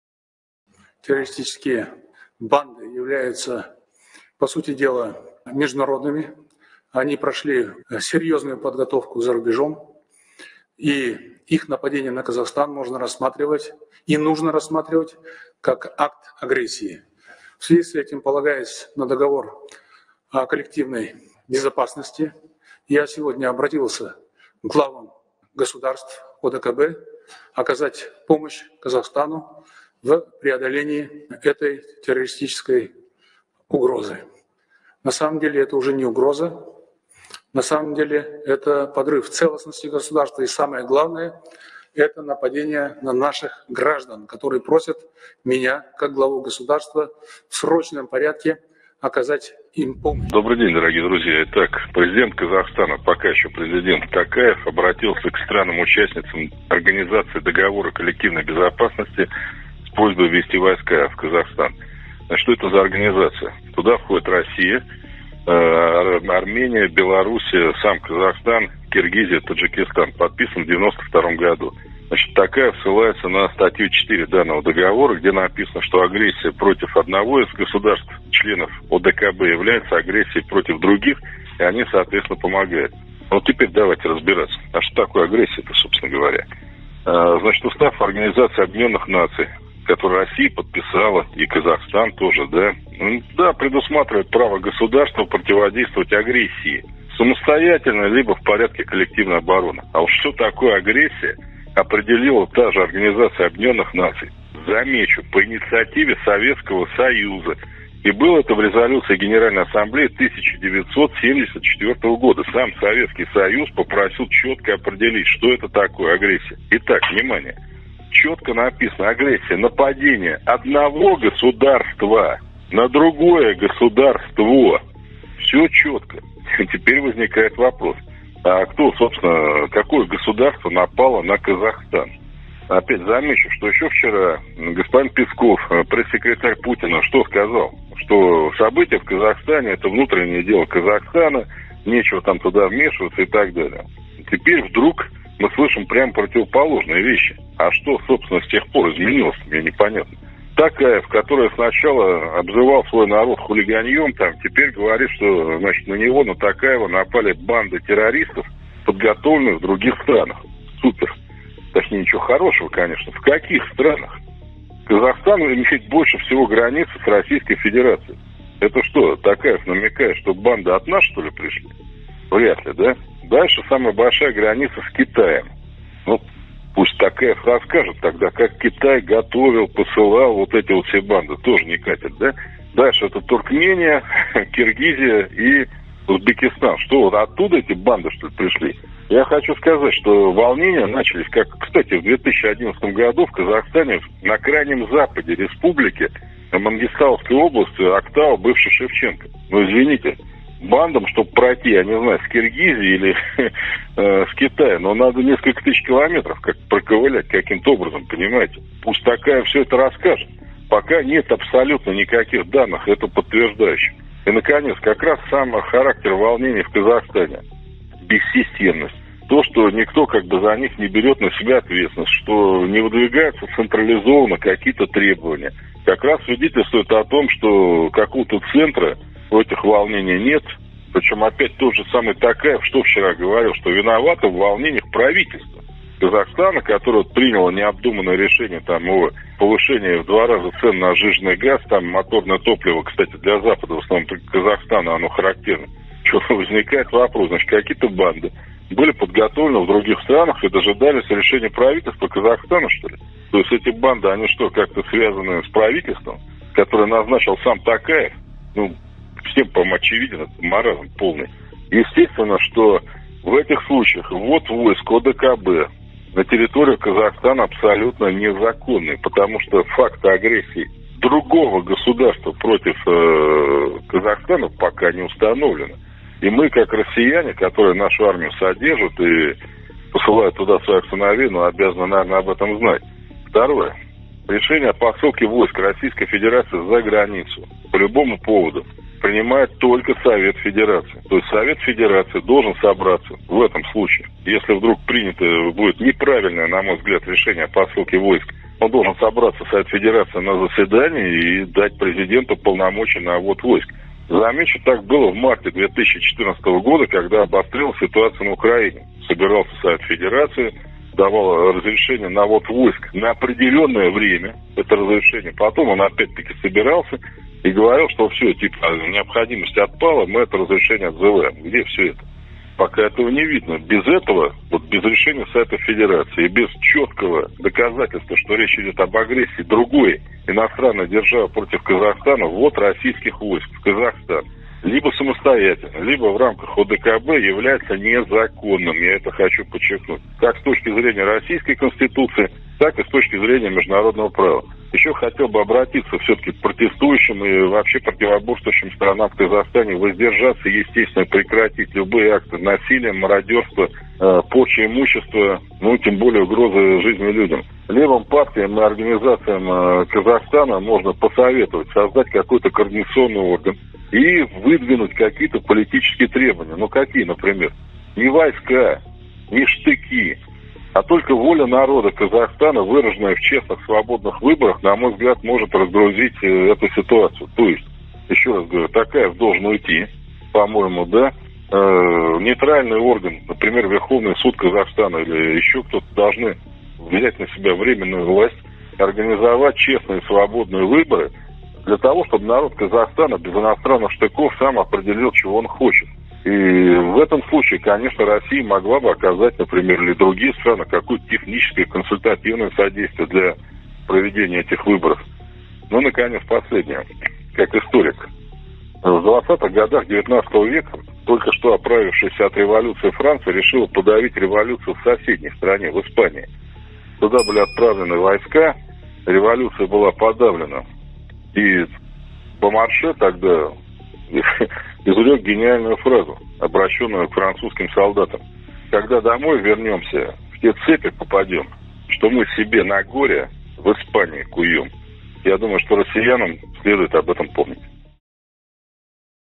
Почему ввод российских войск в Казахстан незаконен?
В ответ на продолжающиеся в Казахстане народные протесты президент республики Касым-Жомарт Токаев обратился к странам Организации Договора о коллективной безопасности (ОДКБ) с просьбой ввести в Казахстан войска для защиты от "террористических банд". Имел ли он на это право и чем это чревато? Объясняет лидер общероссийского движения "За новый социализм" Николай Платошкин